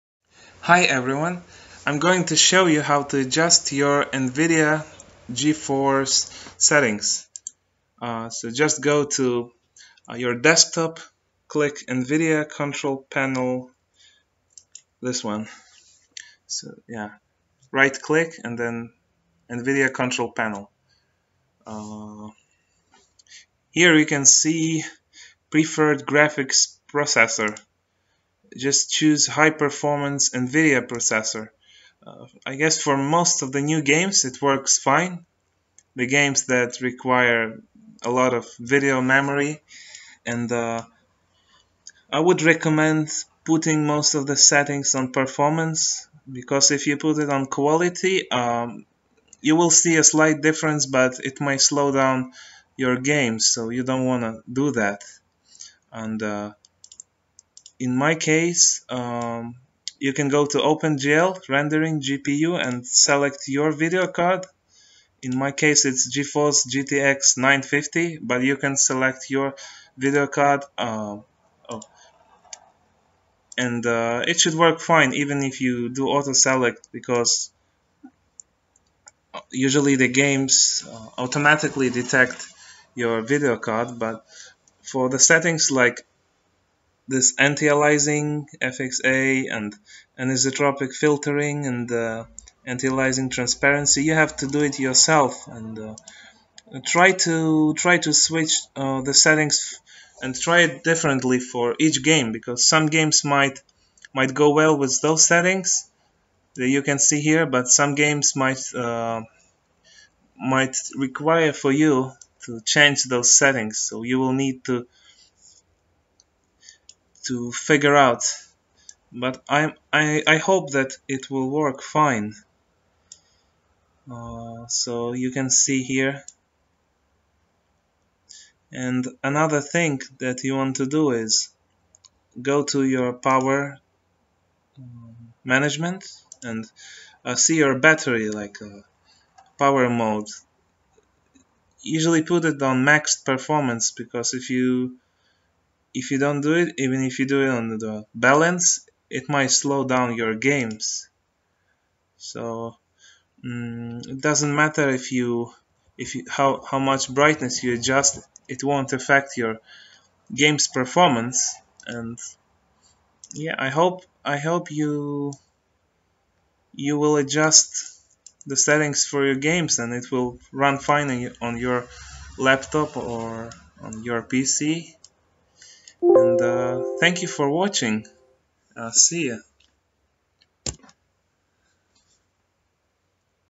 3 Minute Tutorial On How To Adjust Your Nvidia Geforce GTX (950M) Settings On Windows 10
In this video with commentaries you may find a way to fix the slowdowns in the the games that you've played. If you have the same or similar video card to Nvidia Geforce GTX 950M, you might try this settings. Feel free to make various adjustments when needed for a specific video game.